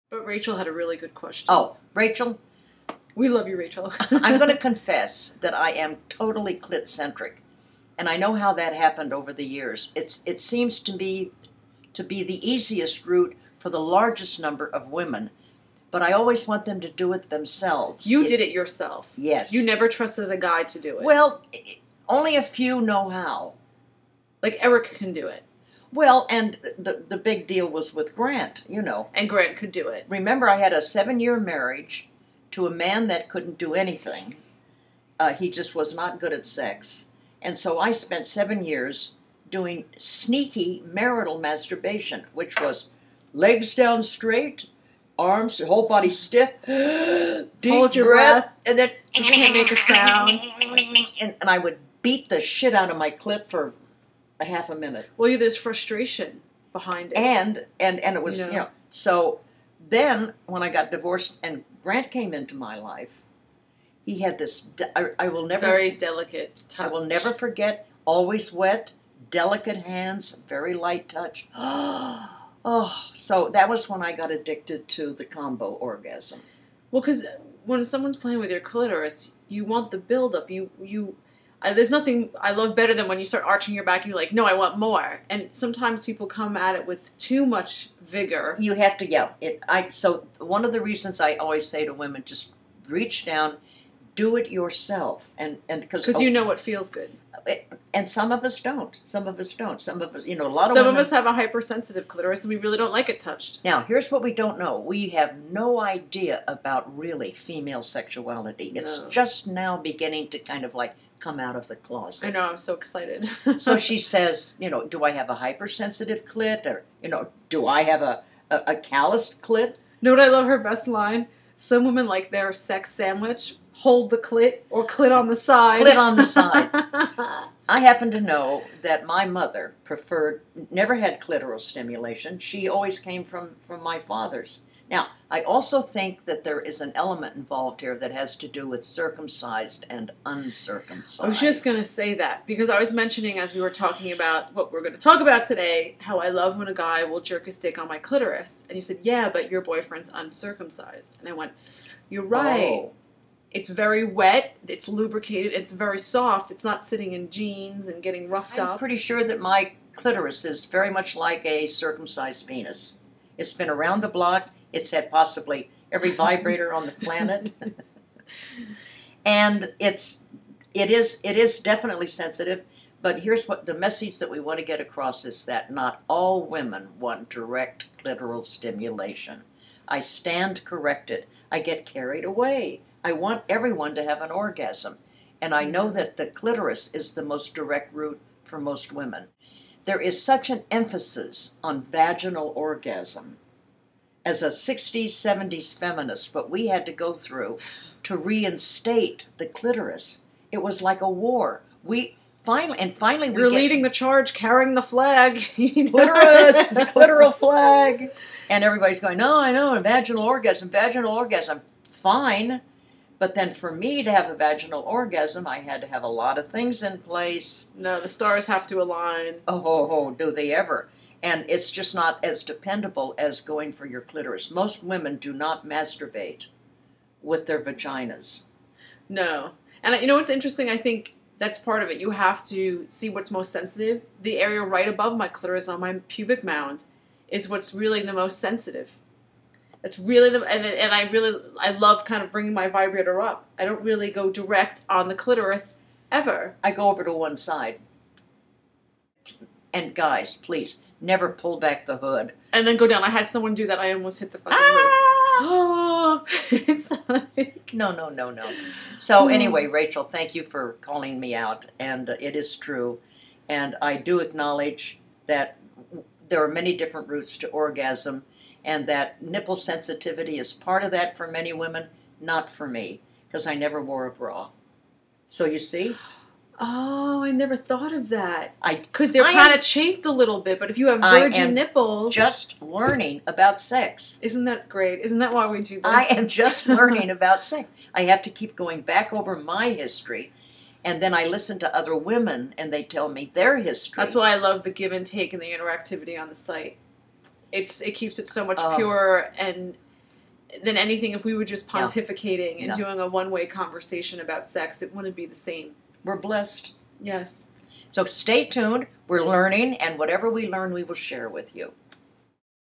Some Women Like Their Sex Sandwich Clit on the Side
We stand corrected - we're clit centric but some women don't like direct clitoral stimulation.